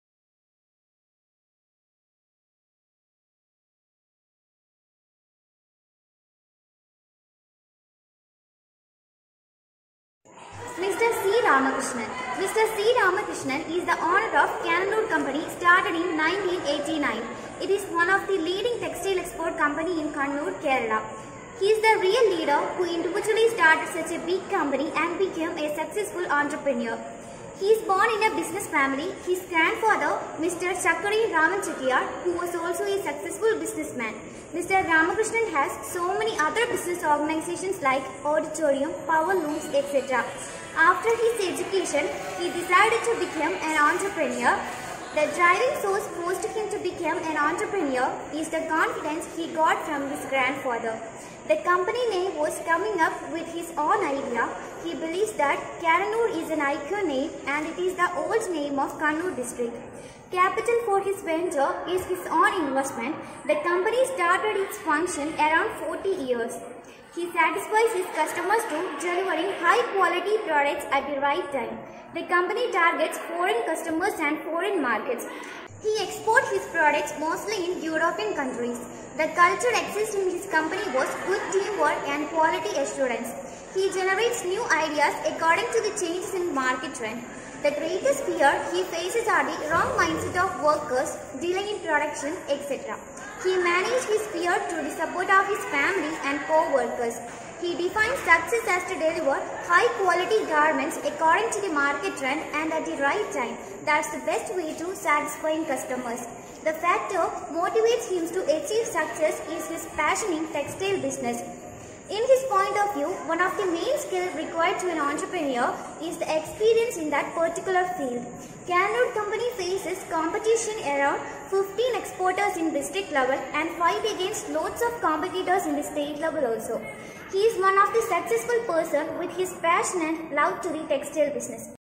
ASBM MBA : Entrepreneur Success Story : Mr C Ramakrishnan, Cannanore Company : Mega K I MBA 2021
ASBM MBA : Entrepreneur Success Story : Mr C Ramakrishnan, Cannanore Company, Kannur Documentary by Mega K I MBA 2021